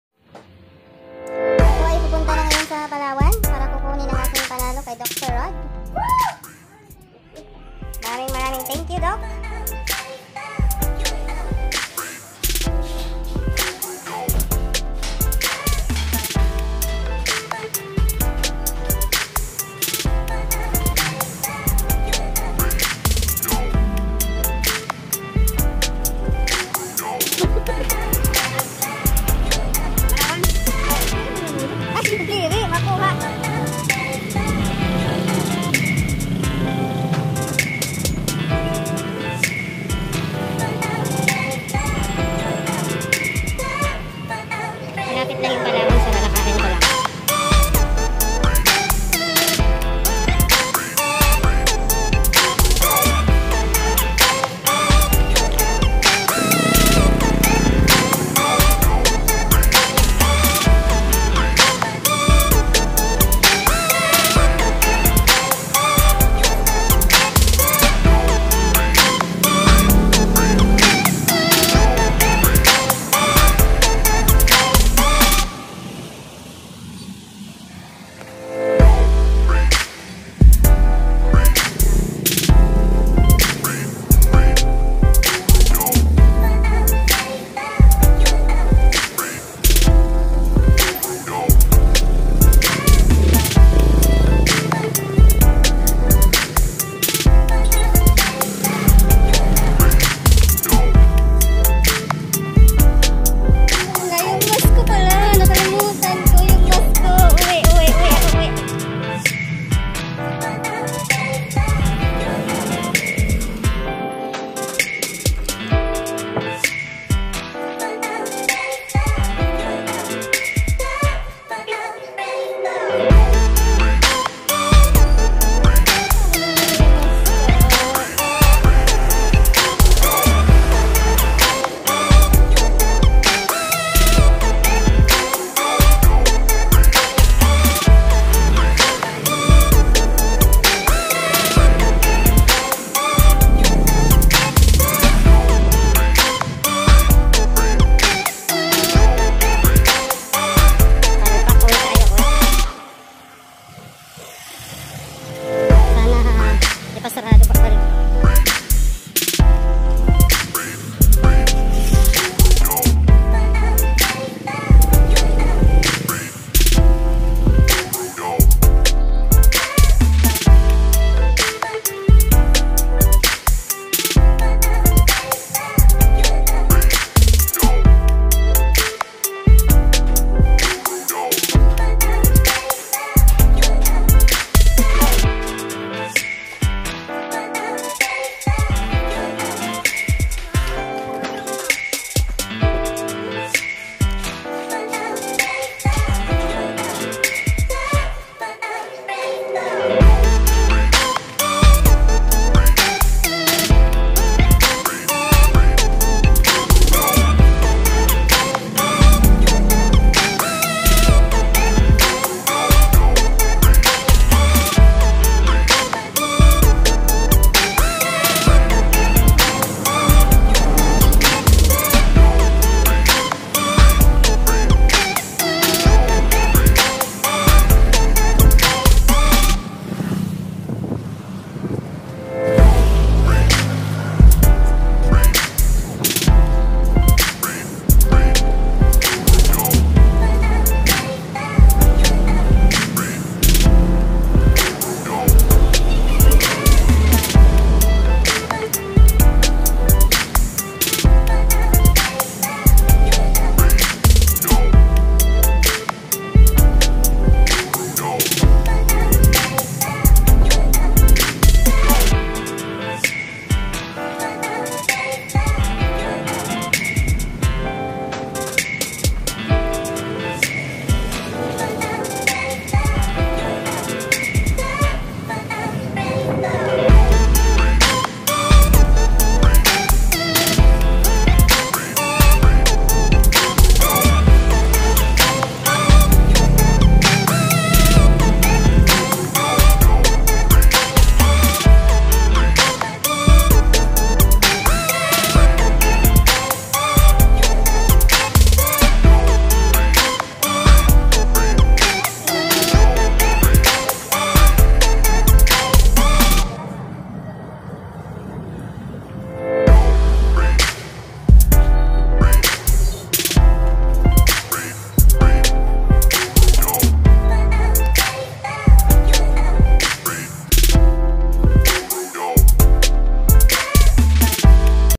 Panalo Kung 20$
August 10,2020
Ang aming butihing kaibigan sa YT
na si
Ay nag pa raffle sa CHANNEL
ni YOLANDATV at nanalo PO
Ako,Isa aq SA dalawang winners
Nung araw na un,kaya thank you so much
Tlaga Dok❤️blessings kapo tlga sa amin
Na nakikipag sapalaran SA mundo ni YT.
Anyways Dok Ang kalahati PO Nyan ay
Gin share din nmin SA aming butihing
Kaibigan din SA YT ganoon din Ang panalo
Sau ni yolanda tv gin share nya rin Ang half
Bsta Dok may PA price o Wala support kami
Sau pangako PO Yan❤️👨‍👩‍👧‍👦👨‍👩‍👧‍👦👨‍👩‍👧‍👦
Amping ka perme dha Dok❤️❤️❤️